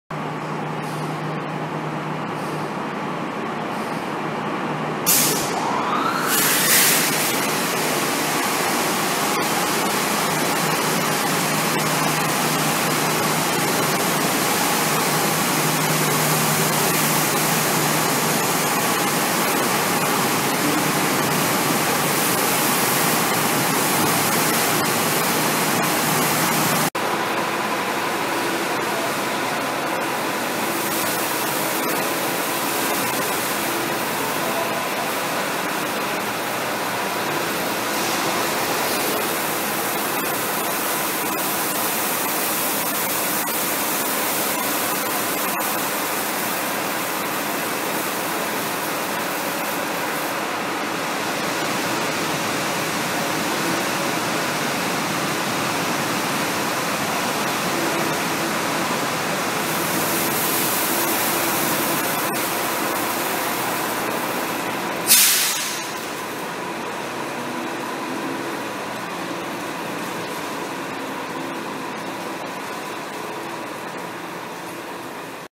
Westmatic 2 Brush Drive Through Bus Wash
Westmatic's 2 Brush Drive Through washing a public transit bus